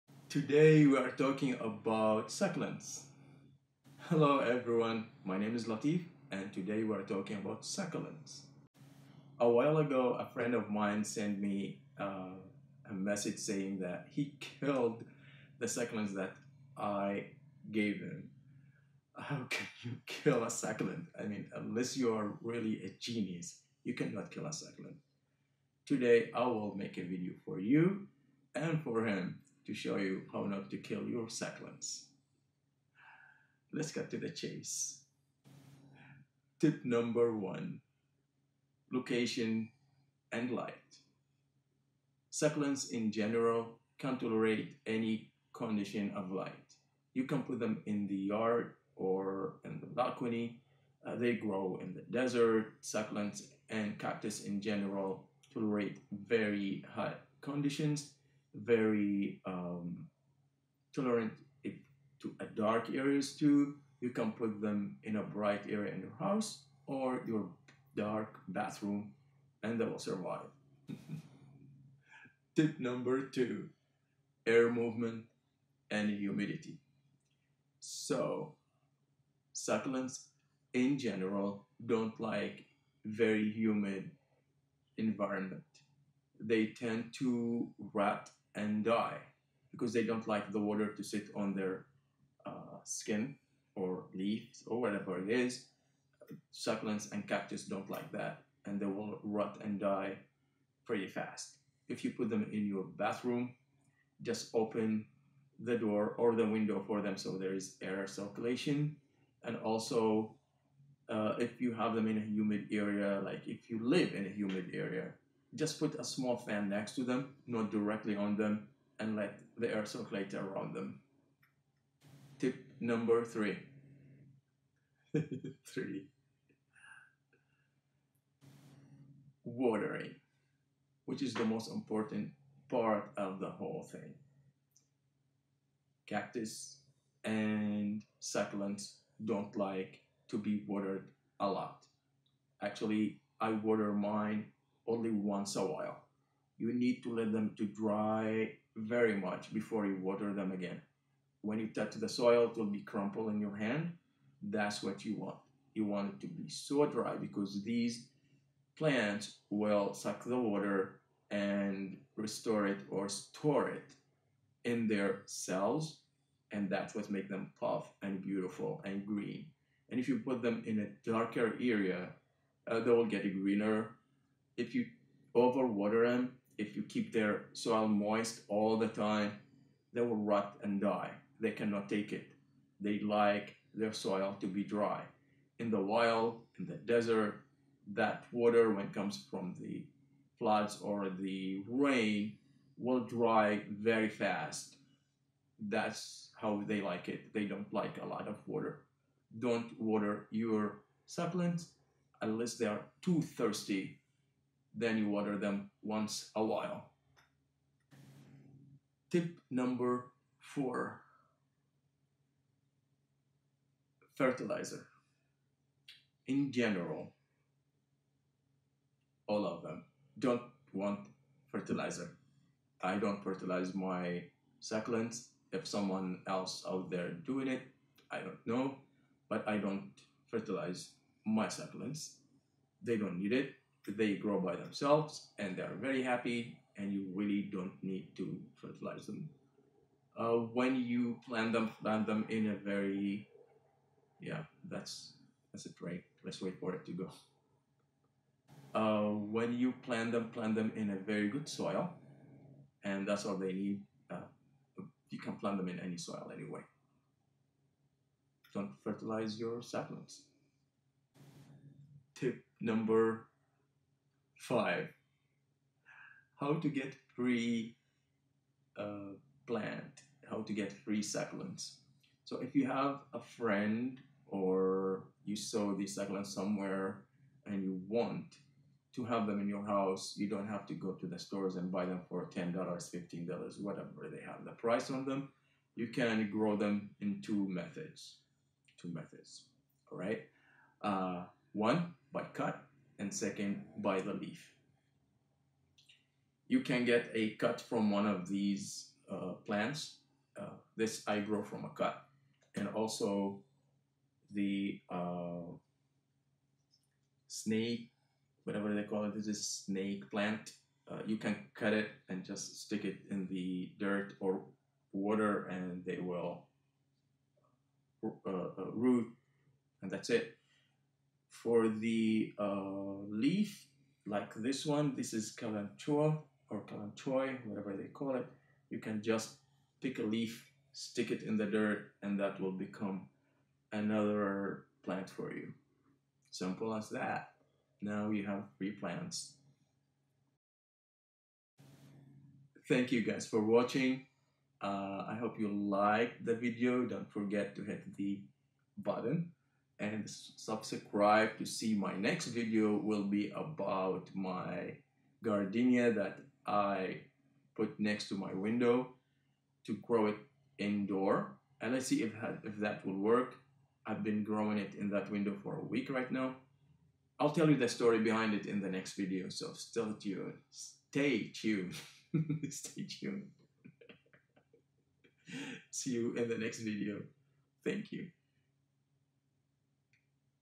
Succulents 5 amazing easy tips to care for succulents Indoor houseplants. How not to kill succulents
Succulents are one of the very low maintenance indoor houseplants and very easy to take care of. But many of us end up killing them. So why they die on us? The answer is in this video. After you watch it you will never kill them again unless you want to.